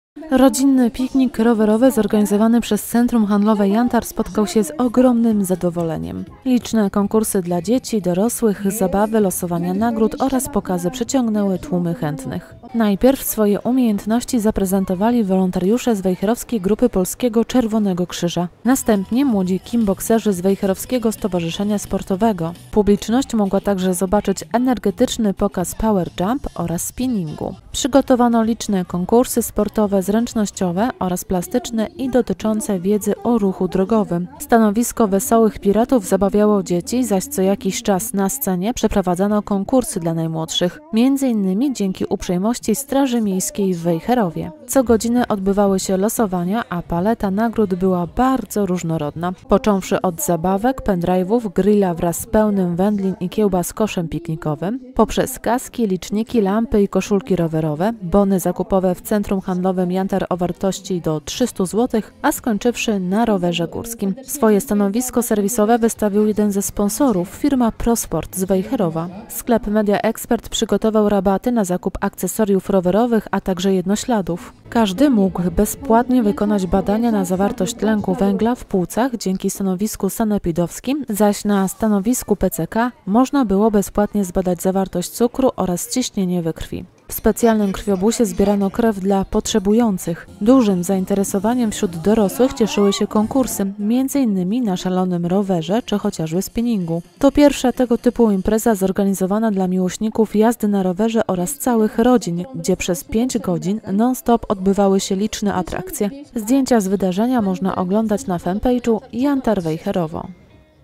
Rodzinnie przed „Jantarem"
Za nami jedna z najwięszych imprez rowerowych w Wejherowie -- Wielki Rodzinny Piknik Rowewrowy zorganizowany przed Centrum Handlowym "Jantar". Liczne konkursy, pokazy, występy oraz zabawy edukacyjne dla dzieci spotkały sie z ogromnym zainteresowaniem